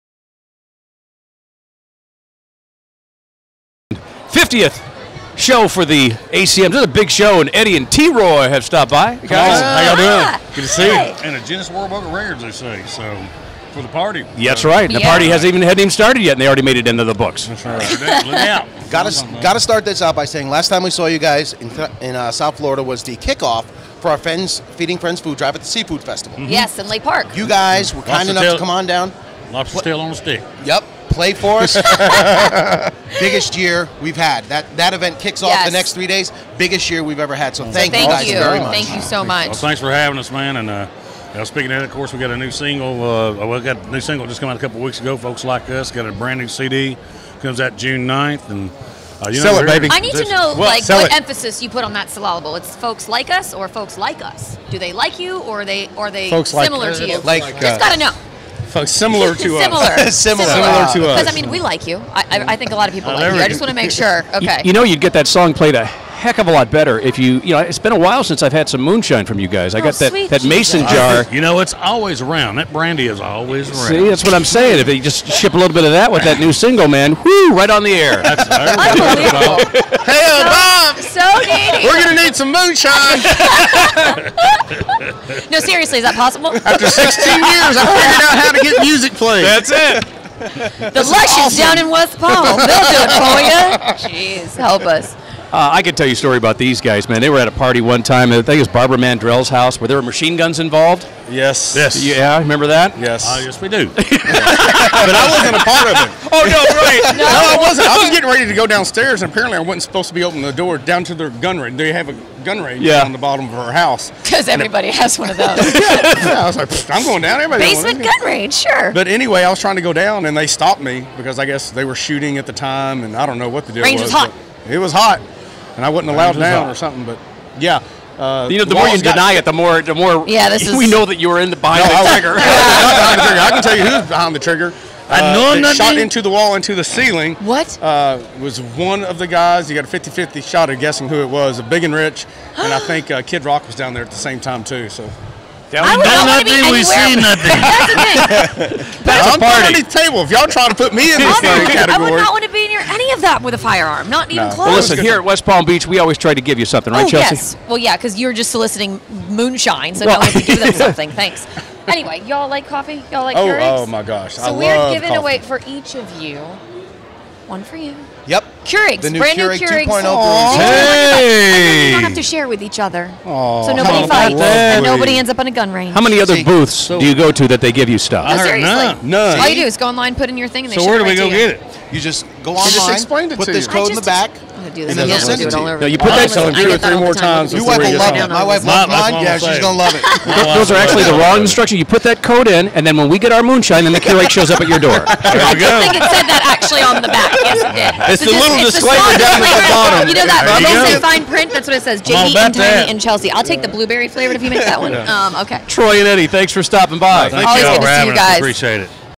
Montgomery Gentry
Montgomery Gentry interview at the ACM Awards.